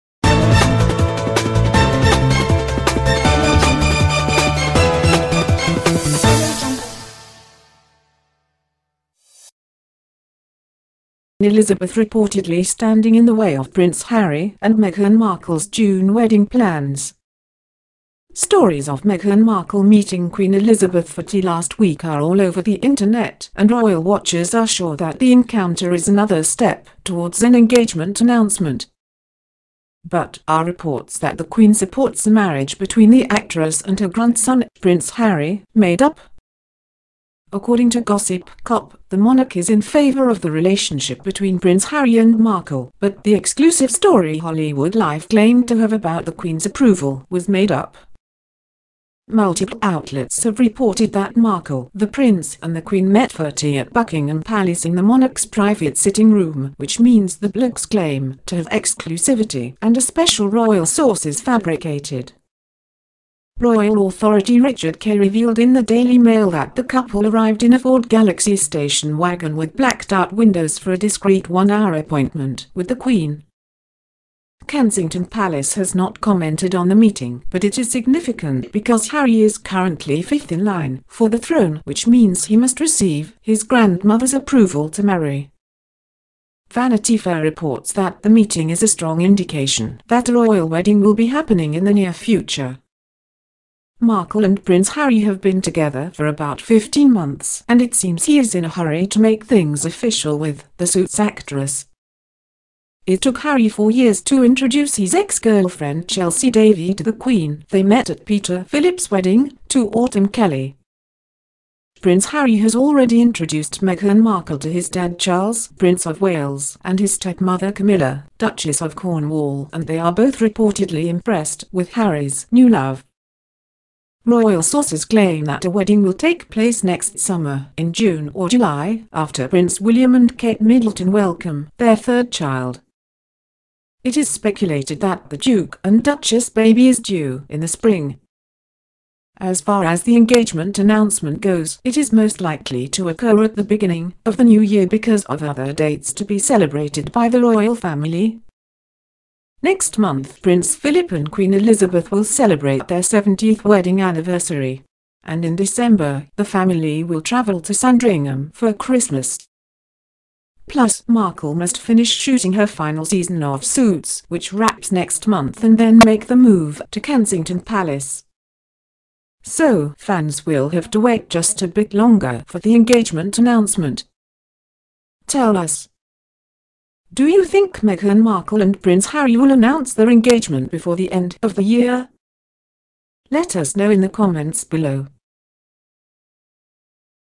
Queen Elizabeth stands on the path of Prince Harry and Meghan Markles plans to marry in June.
Stories of Meghan Markle meeting Queen Elizabeth for tea last week are all over the internet, and royal watchers are sure that the encounter is another step towards an engagement announcement. But, are reports that the Queen supports a marriage between the actress and her grandson, Prince Harry, made up?